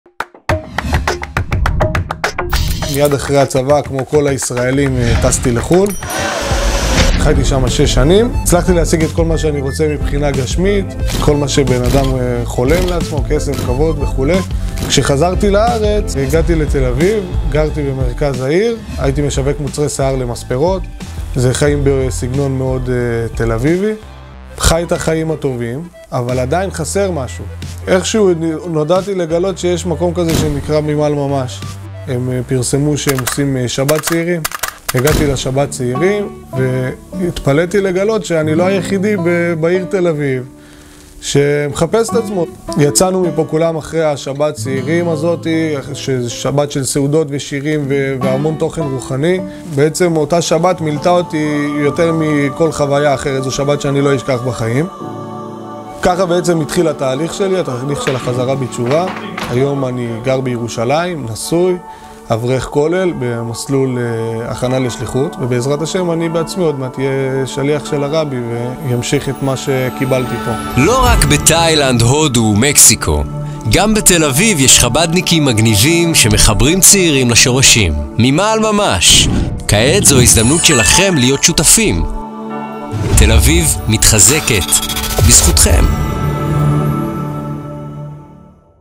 הסיפור של אוהד ▪ ממעל ממש
ממעל ממש בקמפיין מיוחד "מחברים ומחזקים את תל אביב"
20 שנה של פעילות בעיר הכי מיוחדת שיש.